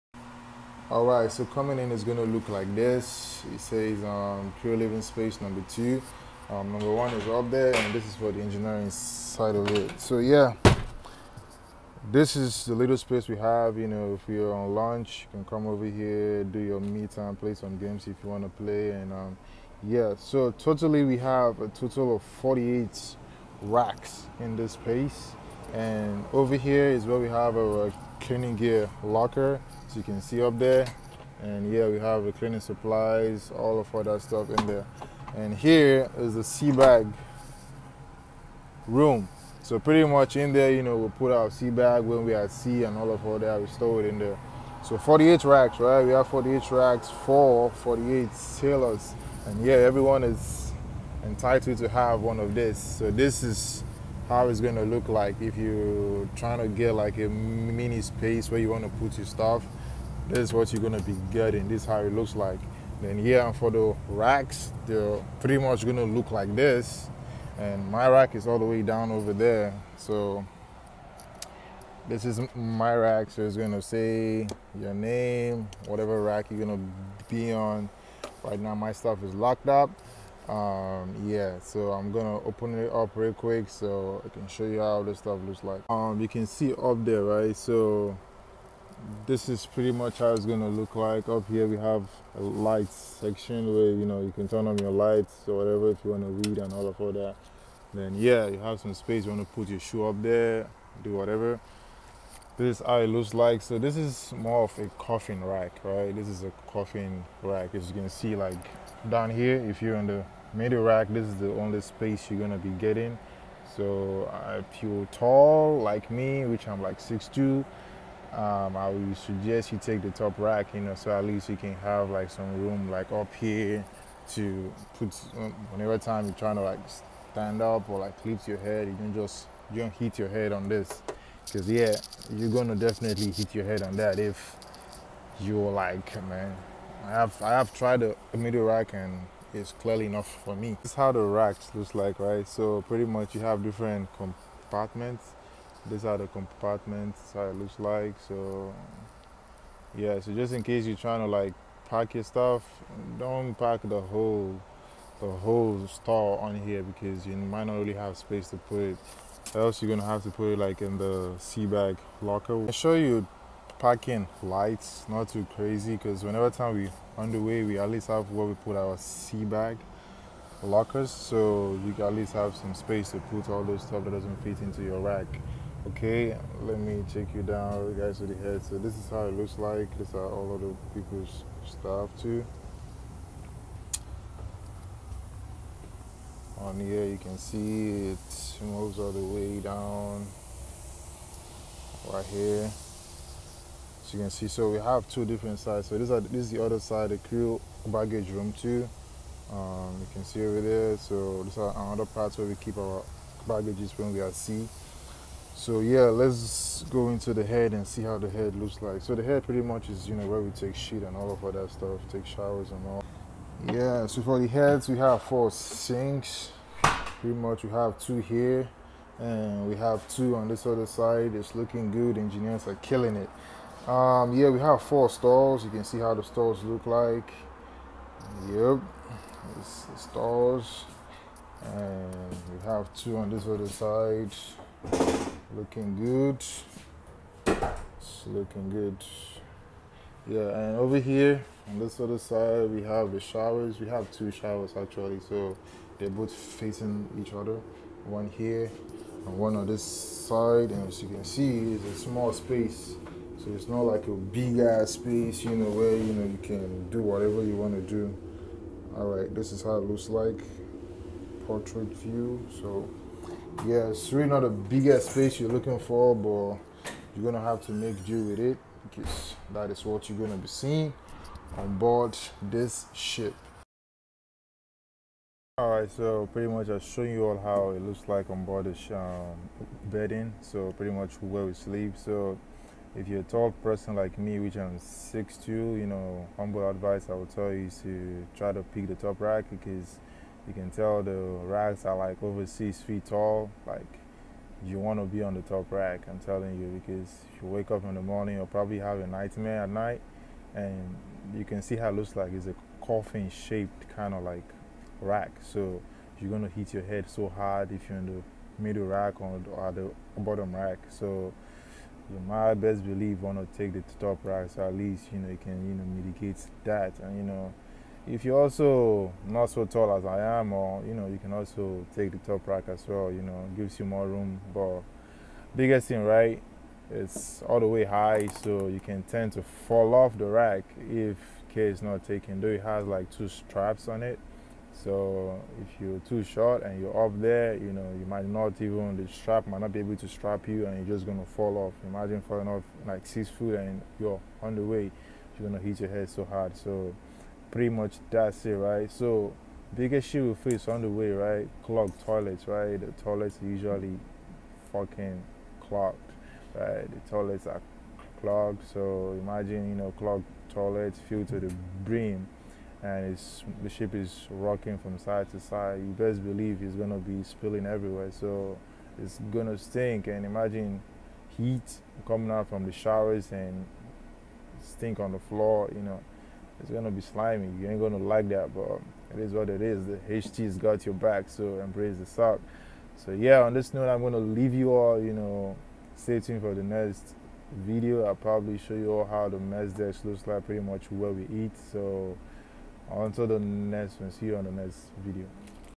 Navy Ship (Destroyer) Berthing Tour
Life onboard a navy ship in real time. Hope this video answer some of your curios question.